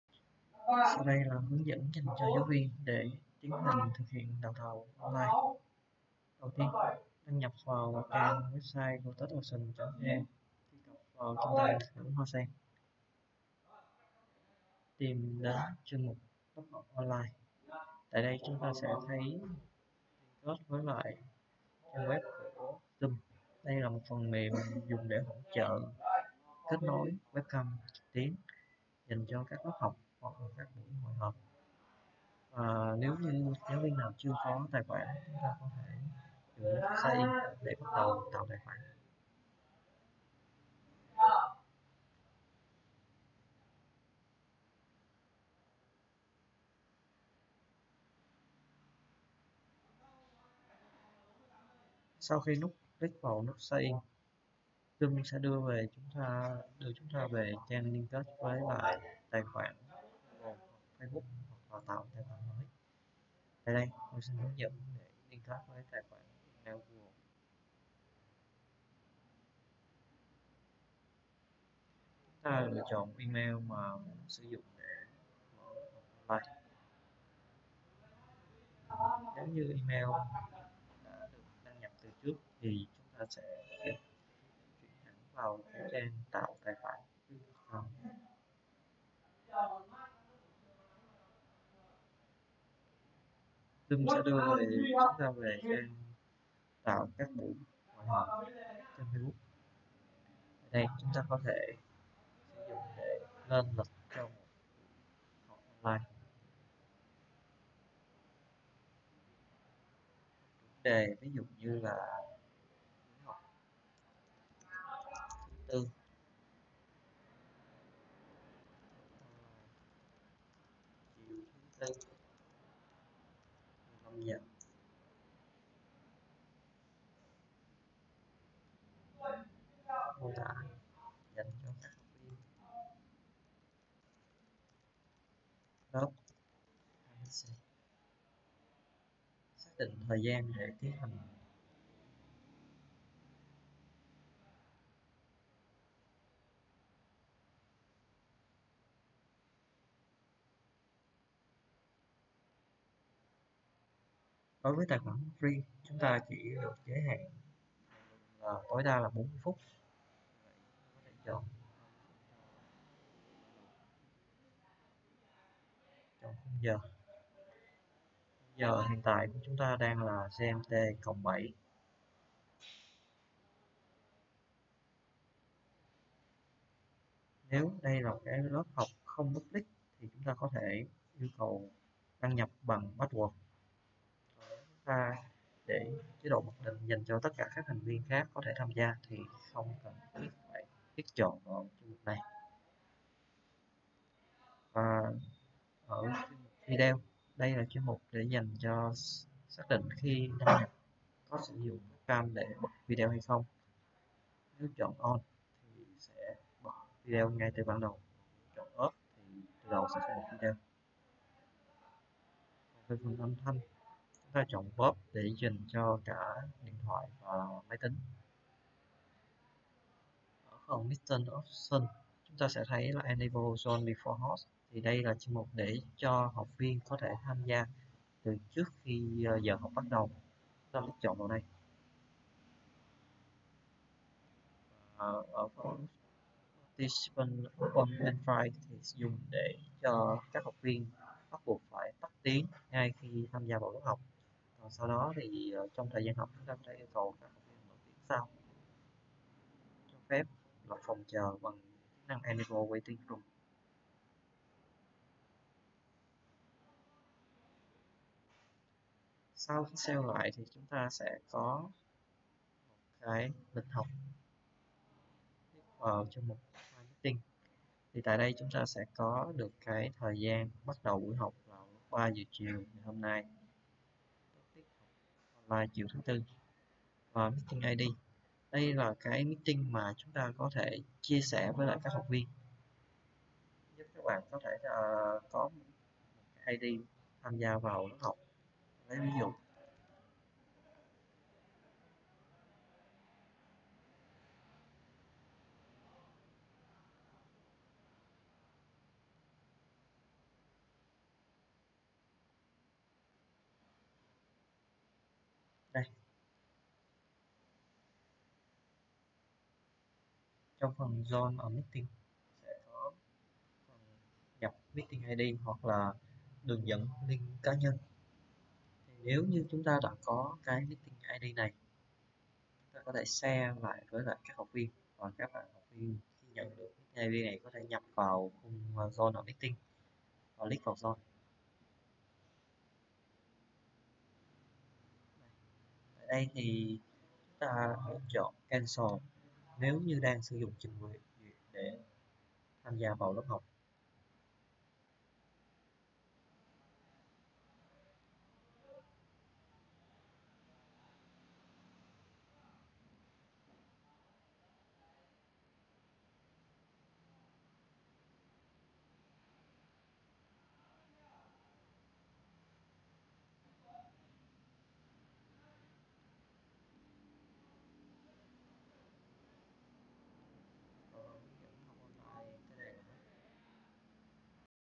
Hướng dẫn giáo viên tạo tài khoản Zoom
Tài khoản Zoom dùng cho chương trình đào tạo trực tuyến.